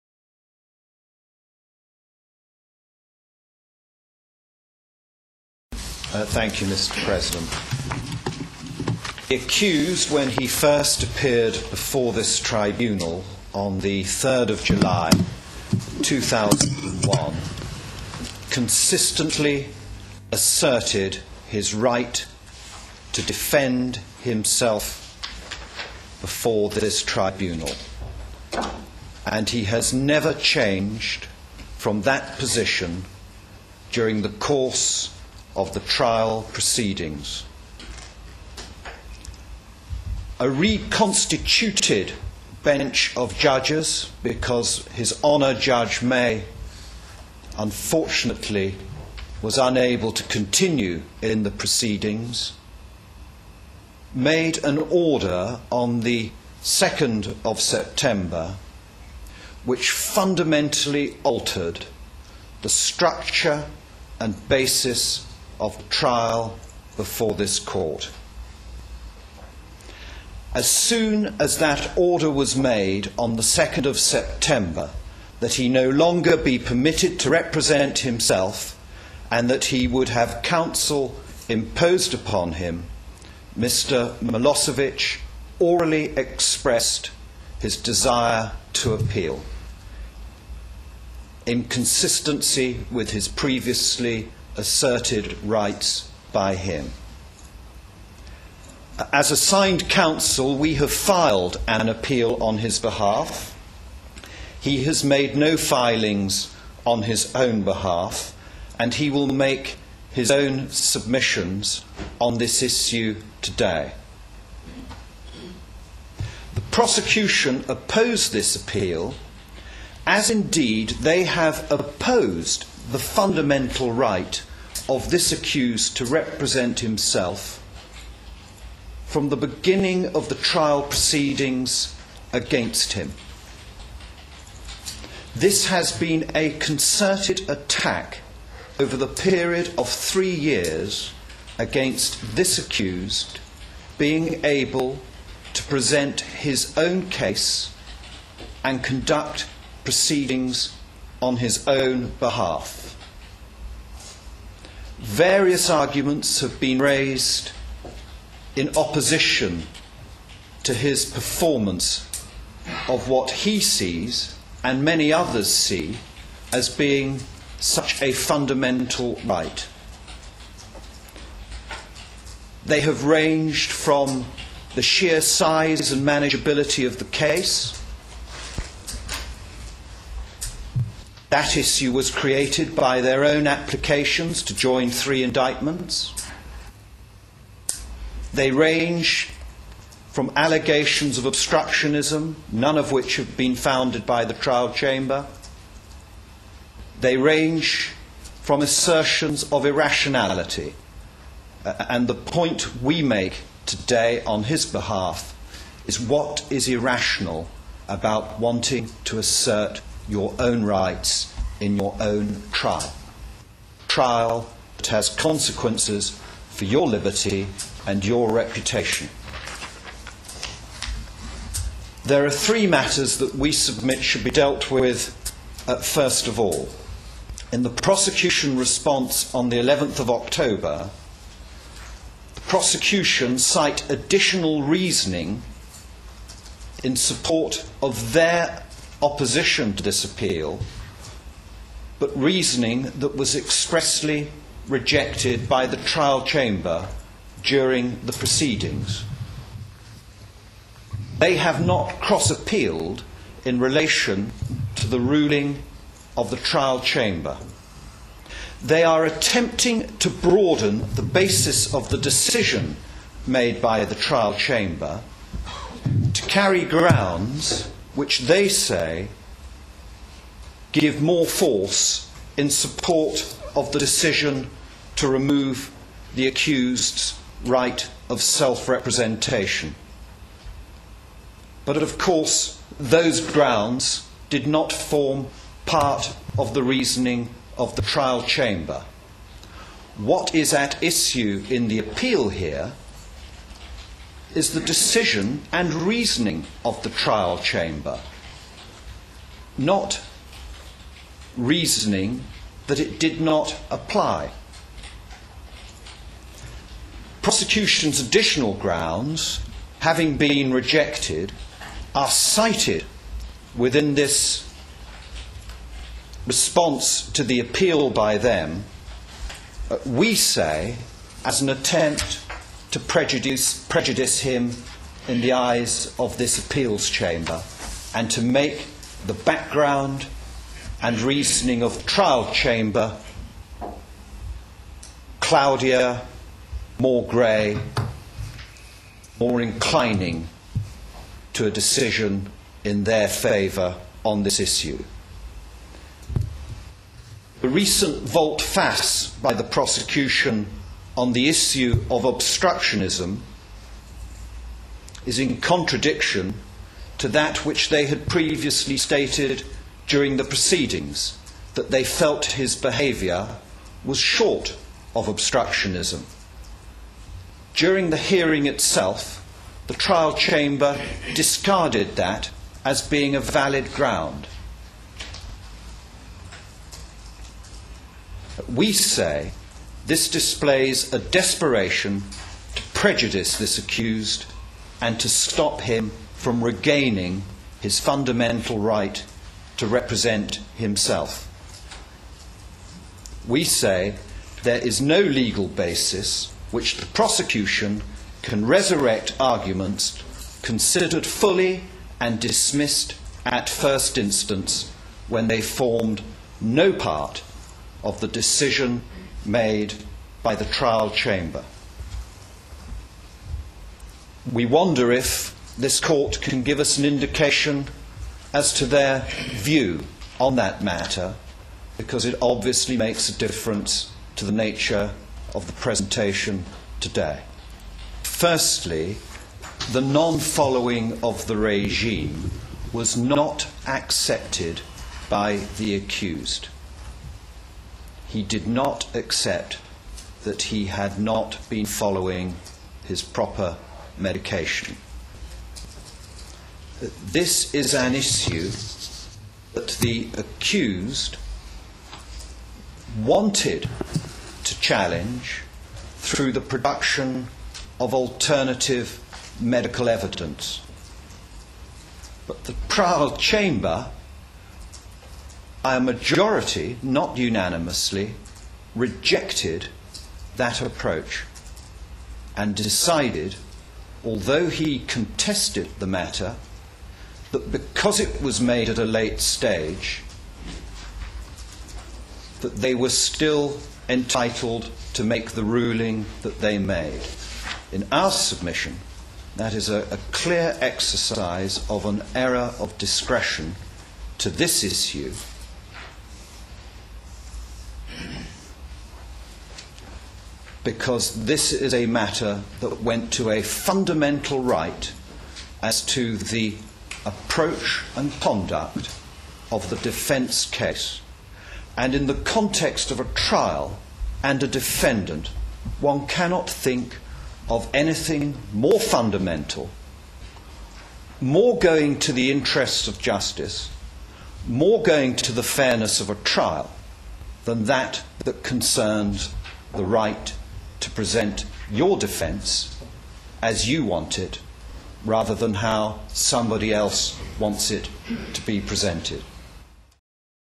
Steven Kay QC: ICTY, Assigned Defence Counsel, The Prosecutor v Slobodan Milosevic
Steven Kay QC, assigned defence counsel for Milosevic, presents submissions before the ICTY re the right to self representation.